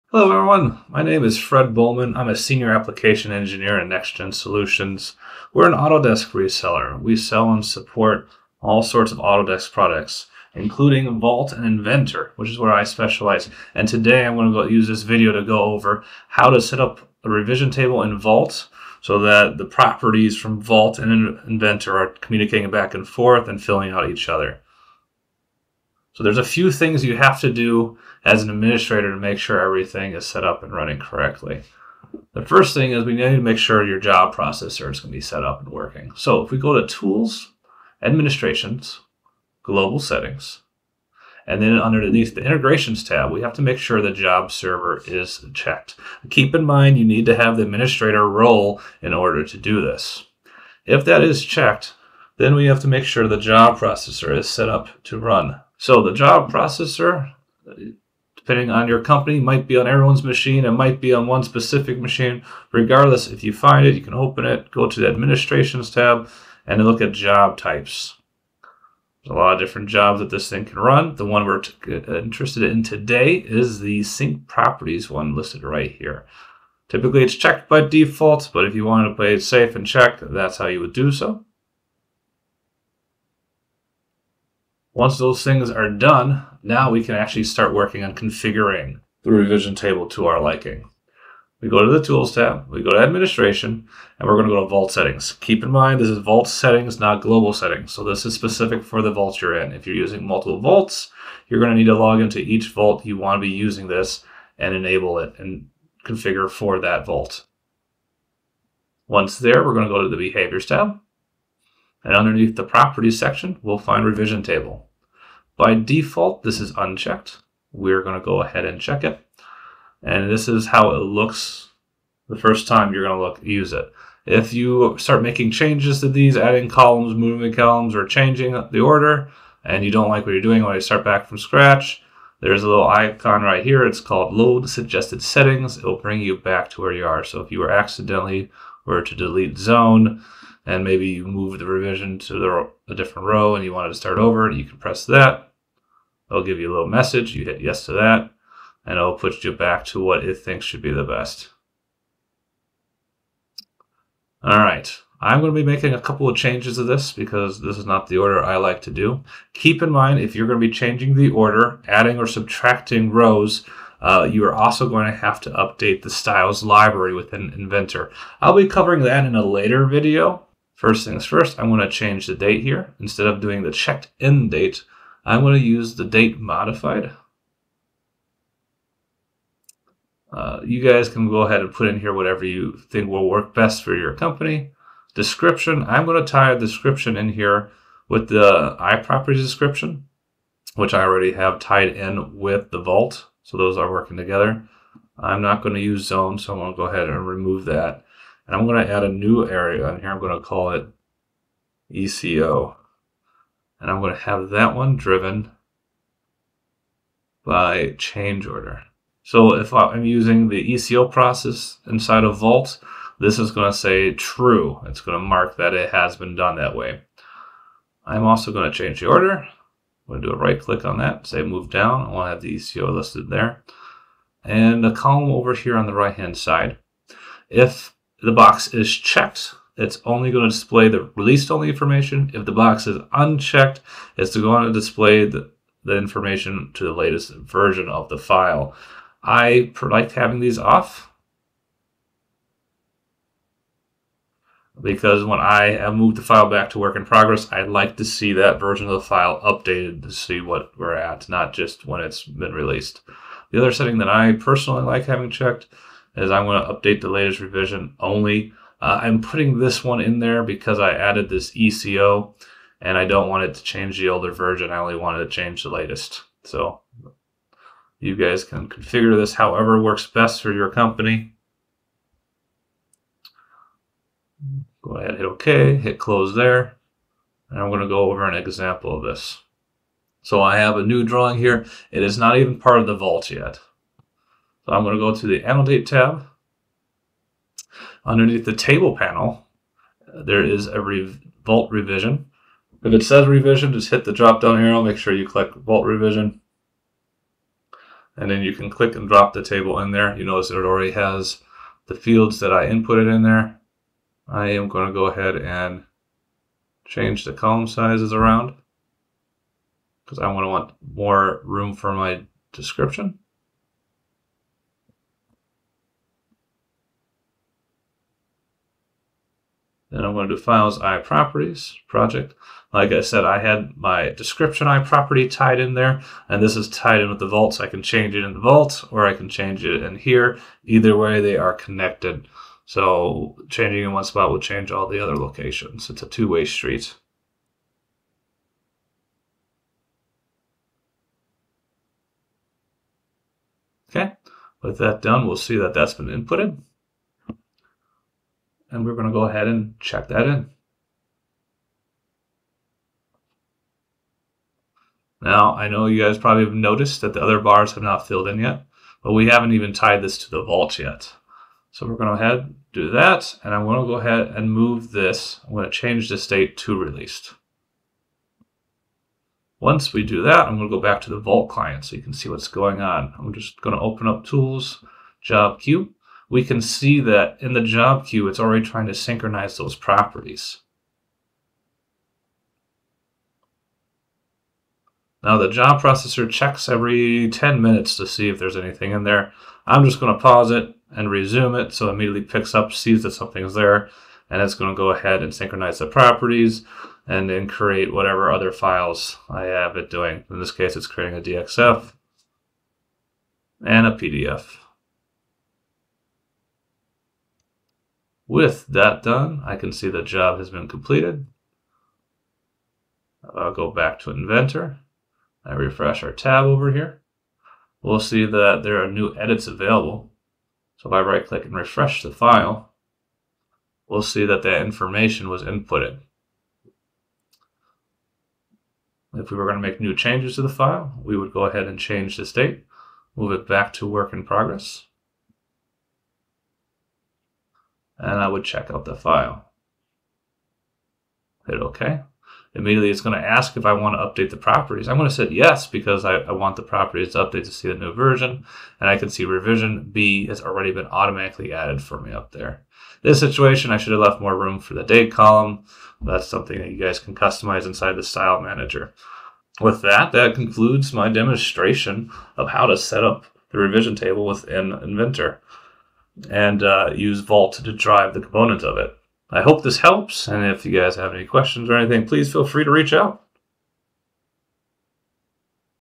How to Set Up Revision Tables in Autodesk Vault
Learn how to create revision tables in Autodesk Vault with seamless communication between properties in Inventor and Vault! Join Fred Bohlmann, Senior Applications Engineer at NexGenSolutions, as he guides you through the step-by-step process of ensuring your design data flows smoothly for efficient Design & Data Management. Don't miss this valuable tutorial to streamline your workflow!

For more information on training classes, go to nexgensolutions.com